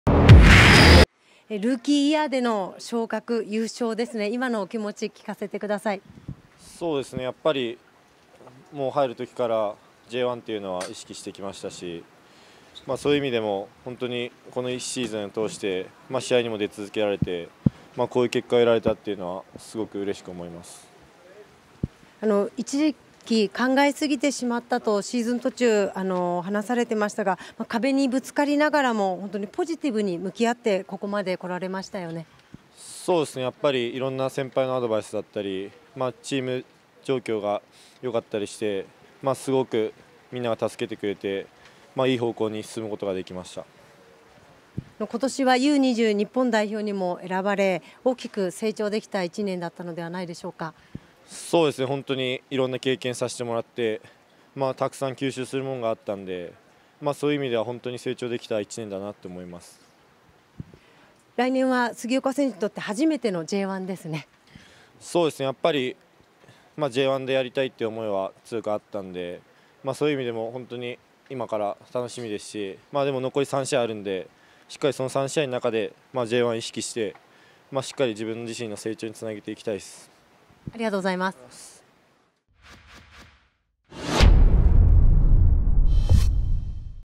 【公式】試合後インタビュー動画：杉岡 大暉（湘南）湘南ベルマーレvsファジアーノ岡山 明治安田生命Ｊ２リーグ 第39節 2017/10/29
2017年10月29日（日）に行われた明治安田生命Ｊ２リーグ 第39節 湘南vs岡山の試合後に収録された杉岡 大暉（湘南）のインタビュー動画です。